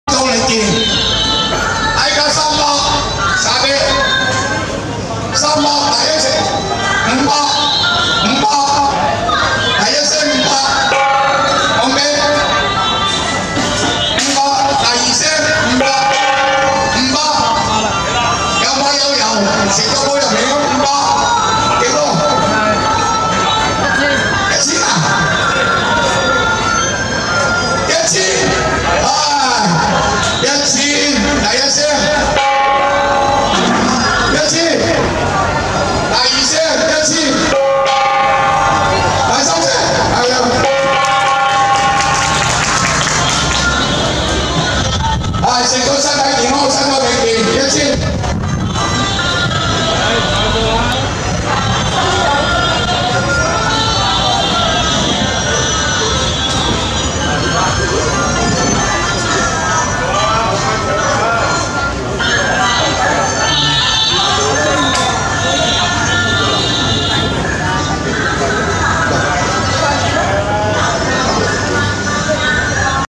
2019潮州南安堂盂蘭勝會︰福品競投１／２
Part 1 of auctions of blessed things on 12/8/2019.